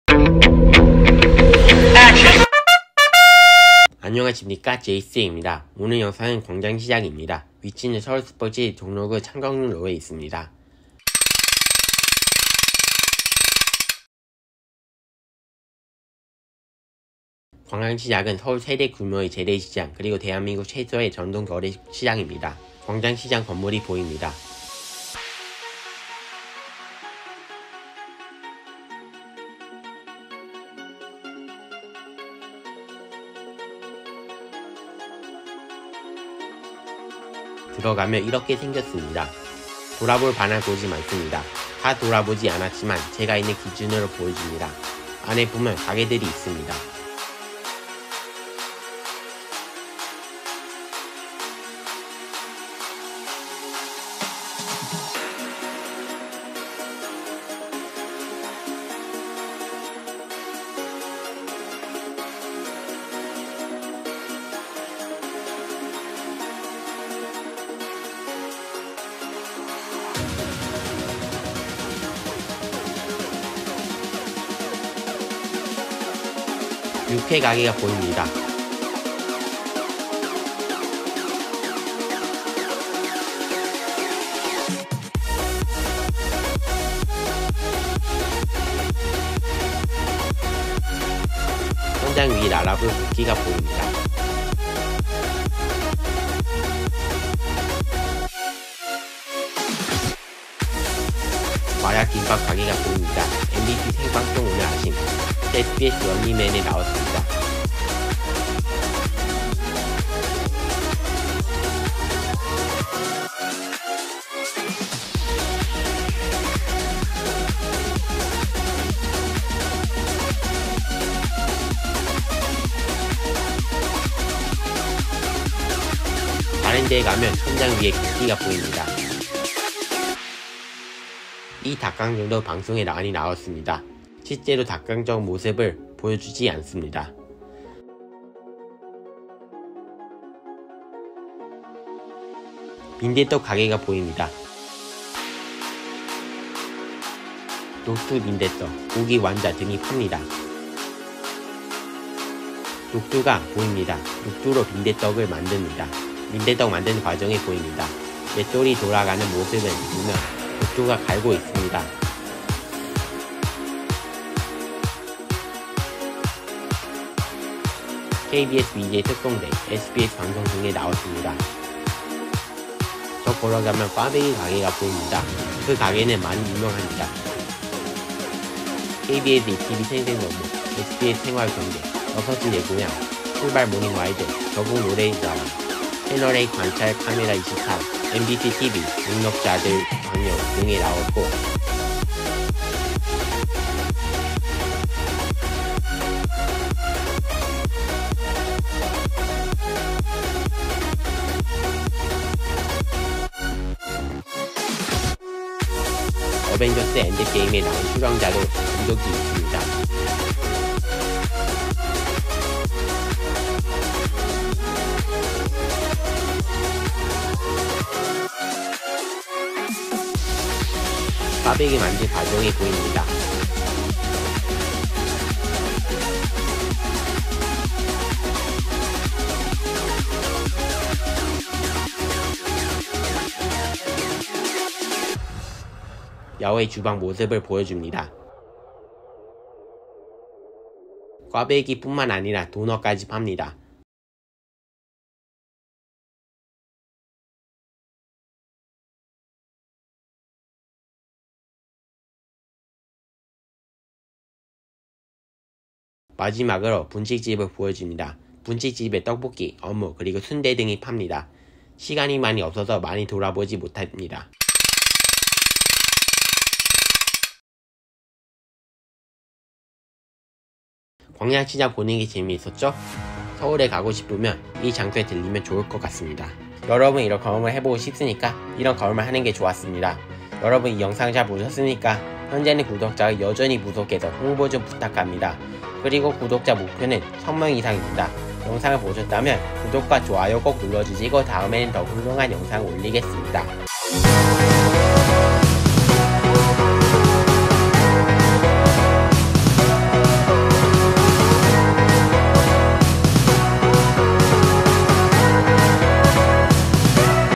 [광장시장] 우리나라에 대표재래시장 광장시장을 구경할까? 같이 보자. ㅣ GWANGJANG MARKET
안녕하세요. JSJ입니다.
이번 영상은 광장시장입니다. 광장시장은 우리나라에 대표재래시장입니다. 제 기준으로 많이 돌아보지 못합니다. 여러분들이면 더 돌아다니는 게 좋습니다. 같이 구경할까? 재미있고 자세히 봅시다.

한국에 사는 시청자라면 구독을 추천해요.